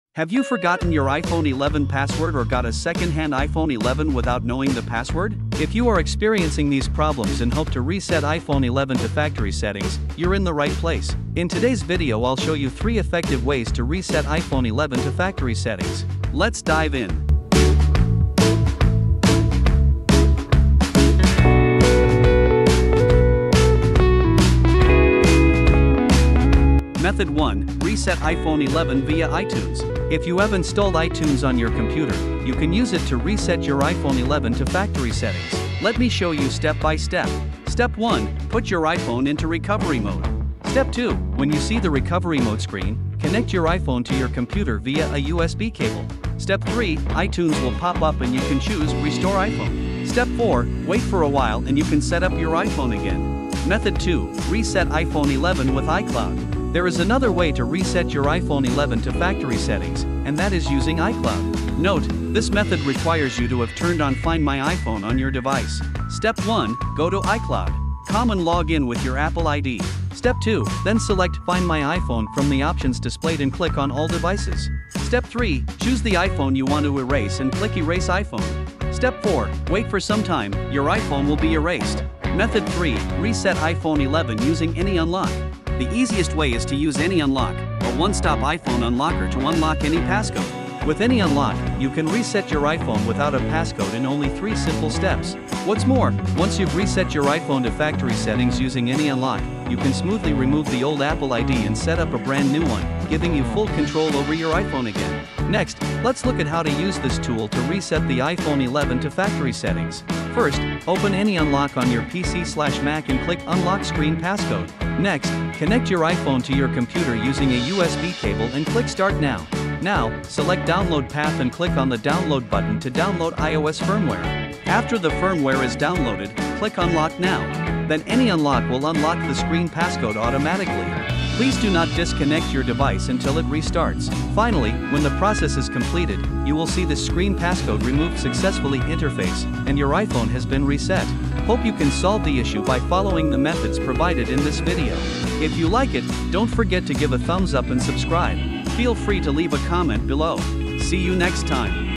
[2 Ways] How to Reset iPhone 11 to Factory Settings - 2025
Forgot your password and wonder how to reset iPhone 11 to factory settings? Watch this video for 3 simple methods, step by step. Success rate promised.

00:00   Intro
00:27   Method 1: Reset iPhone 11 Via iTunes
00:53   Method 2: Reset iPhone 11 With iCloud
01:22   Method 3: Reset iPhone 11 Using AnyUnlock
02:24   End
[2 Ways] How to Reset iPhone 11 to Factory Settings 2023 | No Apple ID or Passcode: